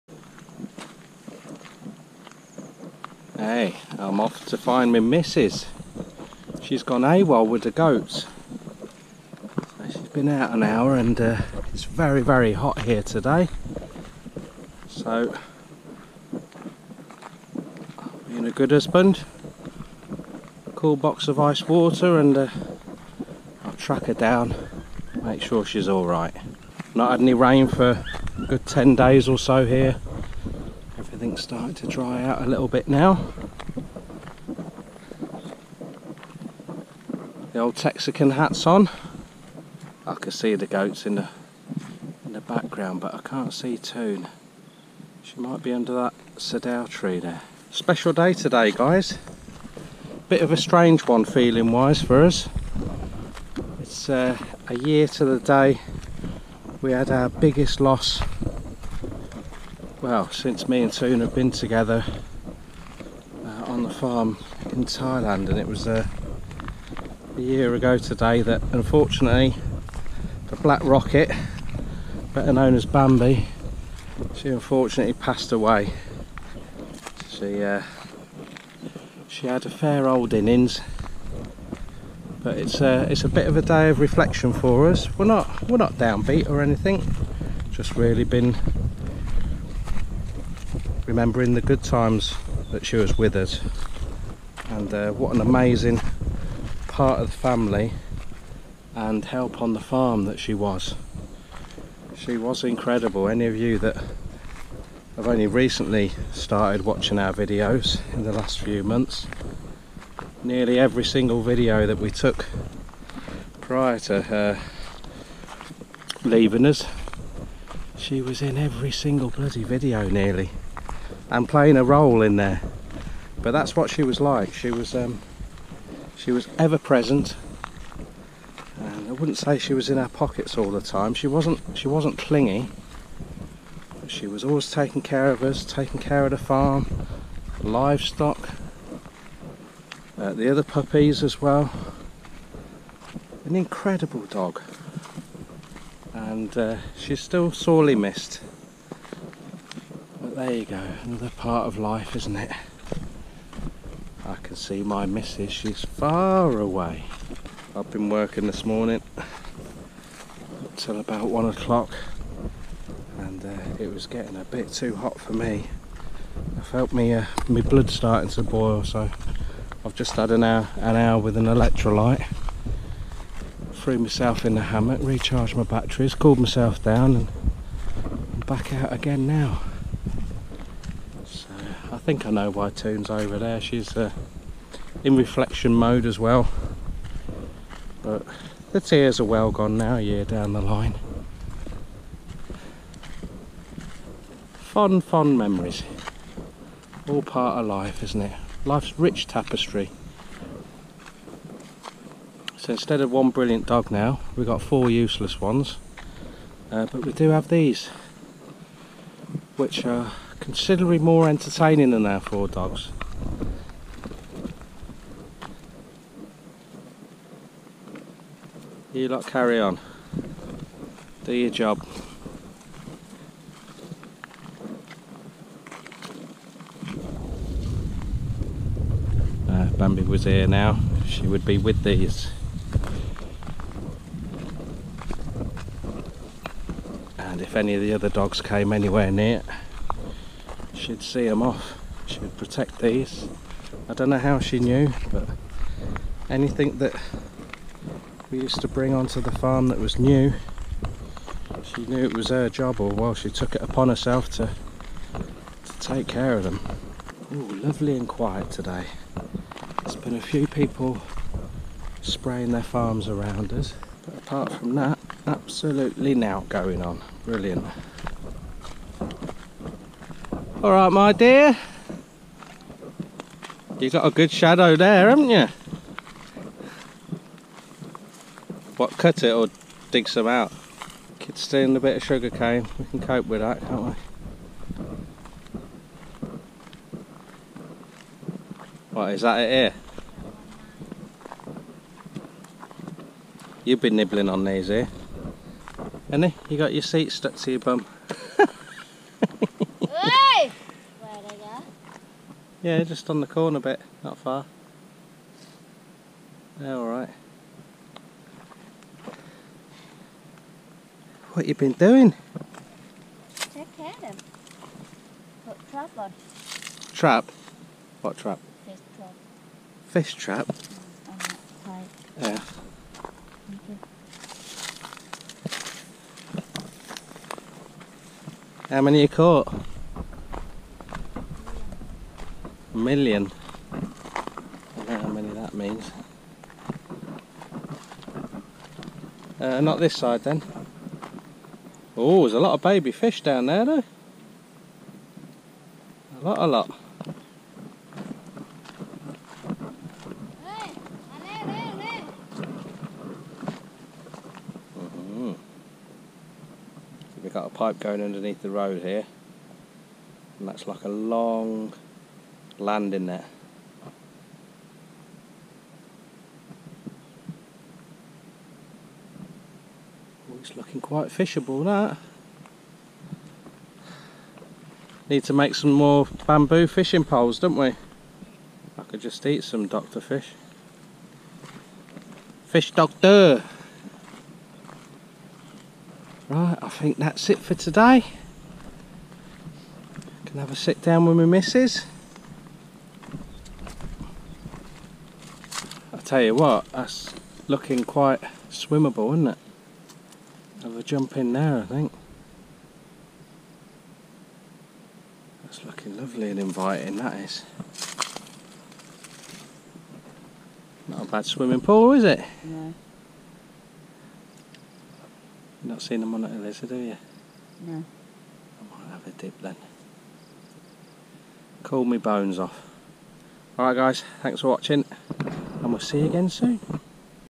Farm Life - Time for reflextion
Today was very much a time to remember someone special to us.
Bambi, aka The Black Rocket.